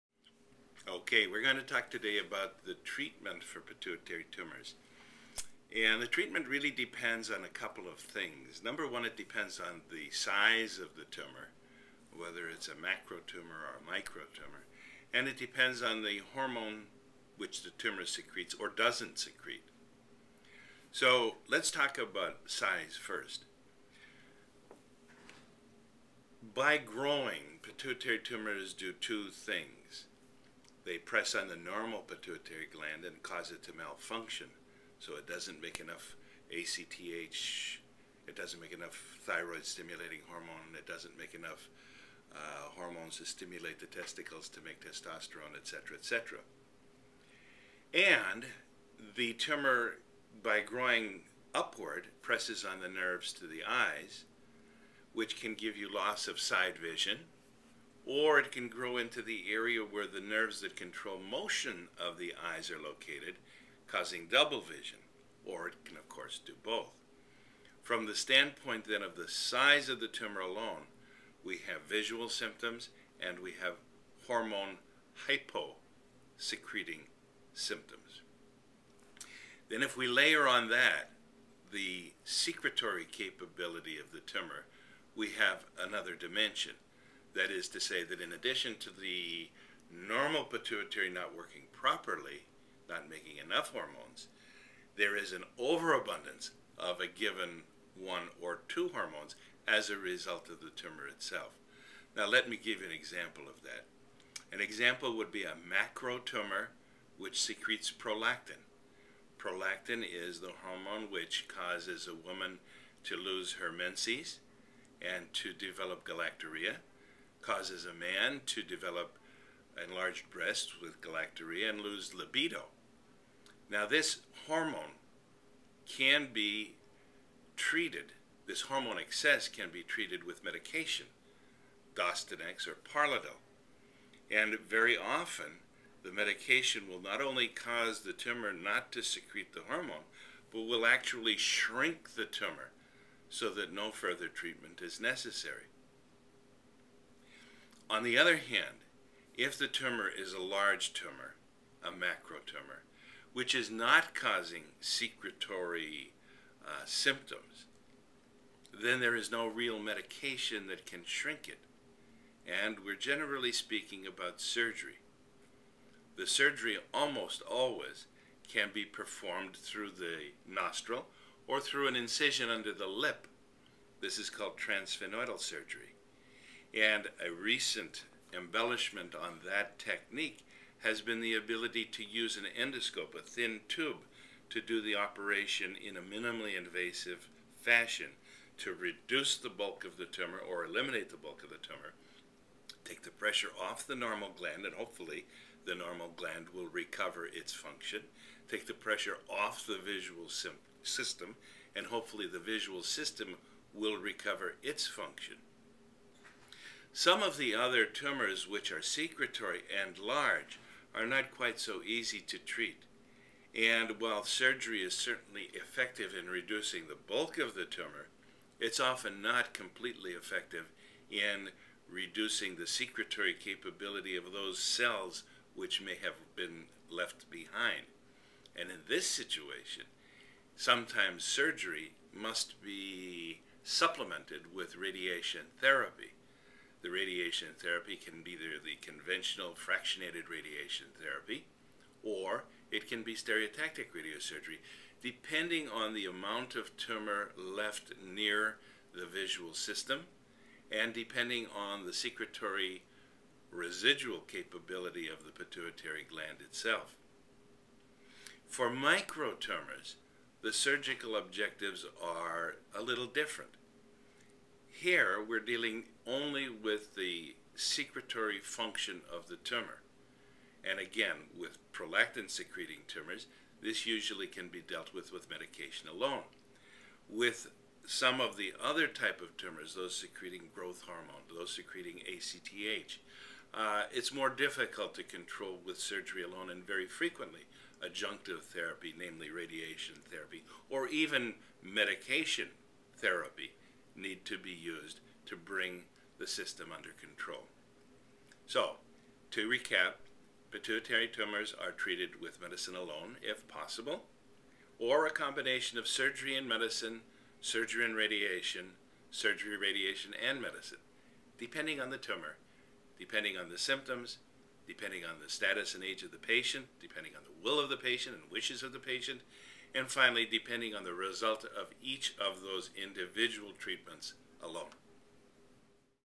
Leonard Cerullo, MD - Discusses Treatment for Pituitary Tumors
As highlighted in a recent New York Times Magazine article, pituitary tumors can be difficult to diagnose due to the vague nature and wide array of symptoms. Their treatment can also present challenges as a result of the distinct differences in the types of pituitary tumors. Size and the secreting or non-secreting nature of the tumor influence the appropriate treatment, either with medicines, surgery and/or radiation therapy or stereotactic radiosurgery. Dr. Cerullo briefly reviews the various types of pituitary tumors and discusses the treatment options available.